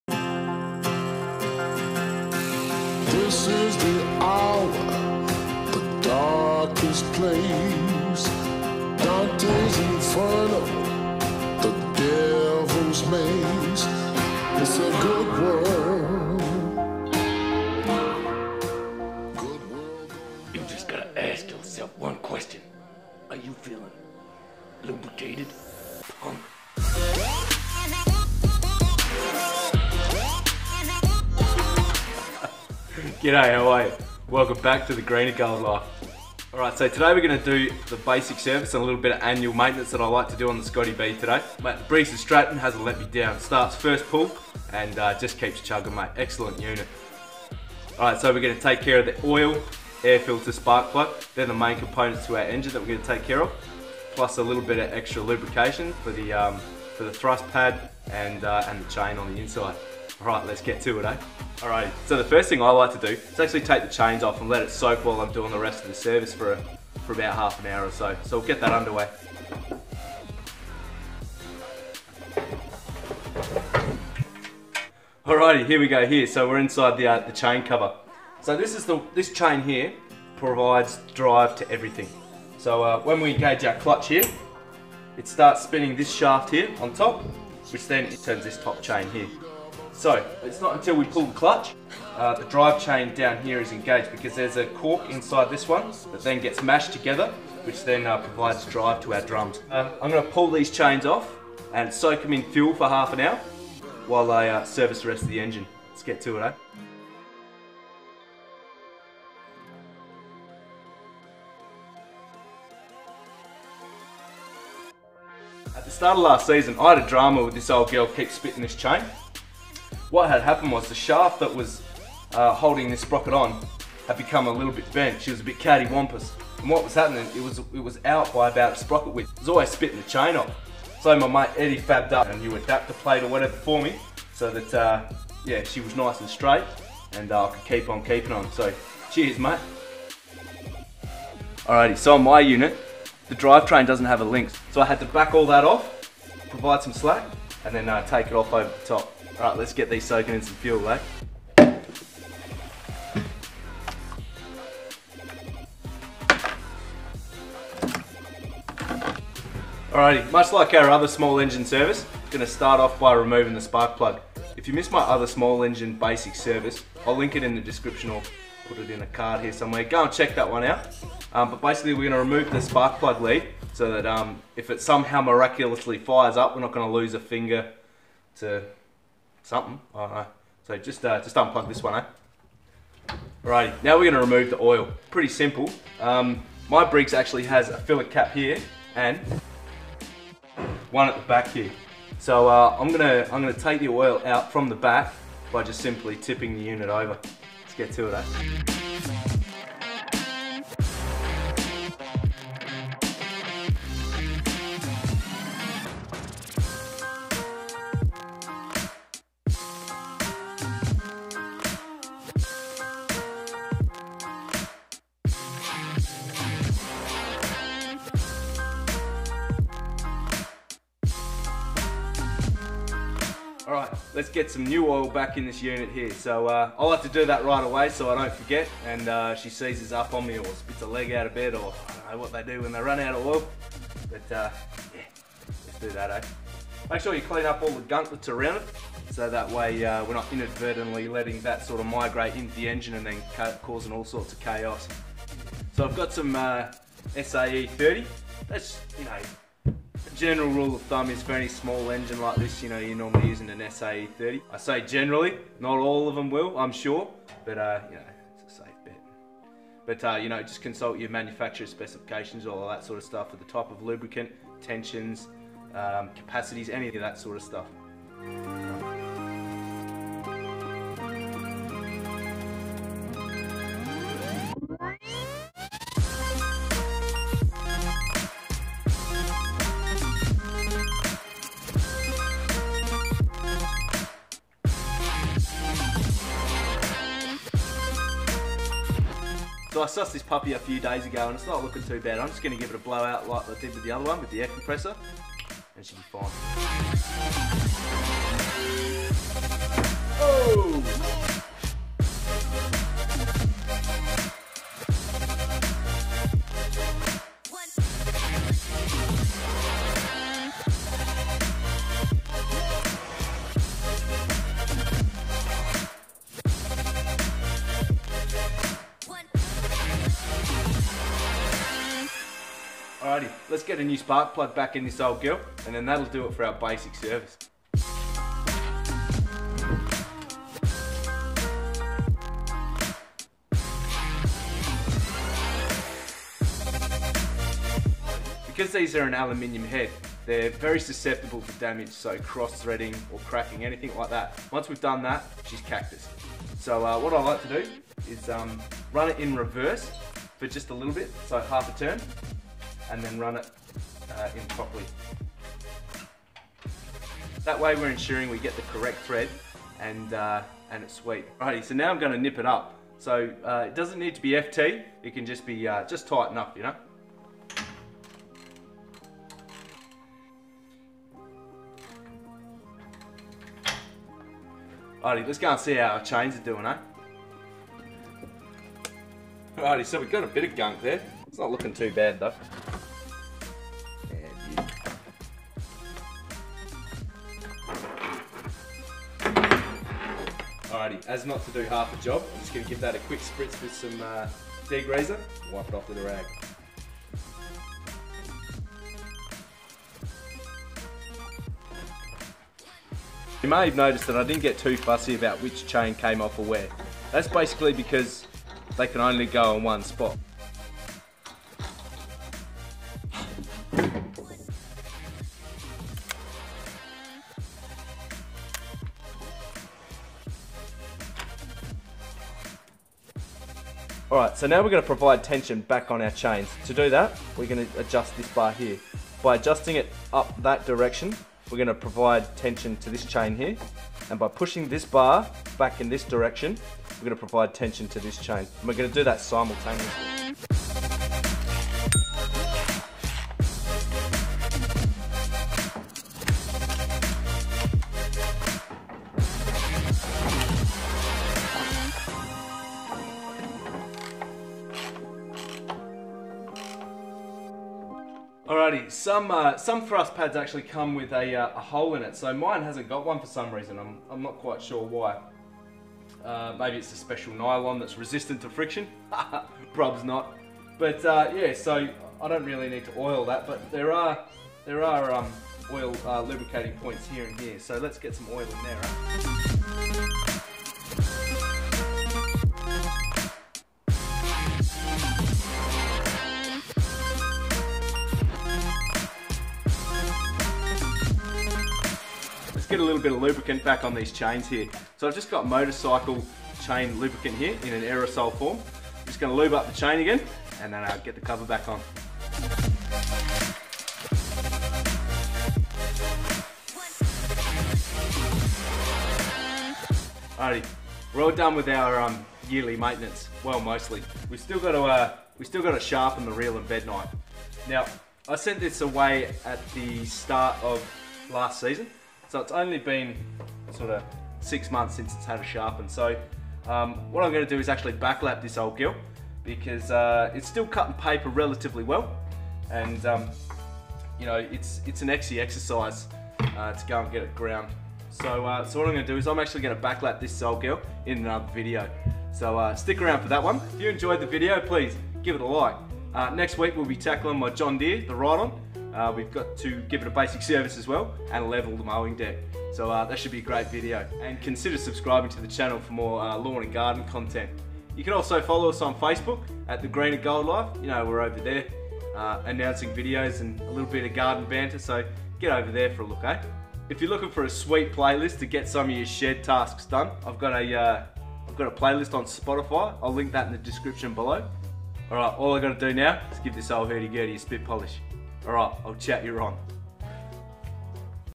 SCOTT BONNAR 45 BASIC SERVICE - 17" winter service
Gday old mate,

Servicing your machinery is an excellent way of assisting longevity of your assets. Doing it your self is as easy as falling off a log! Always remember to remove the spark plug lead prior to undertaking works on your Scott Bonnar.

You can get some degreaser here (Amazon)

You can follow The Green and Gold Life on Face Book

If you are looking for a sweet spotify Play list while your putting in the hard yakka

Righto Ill chat ya ron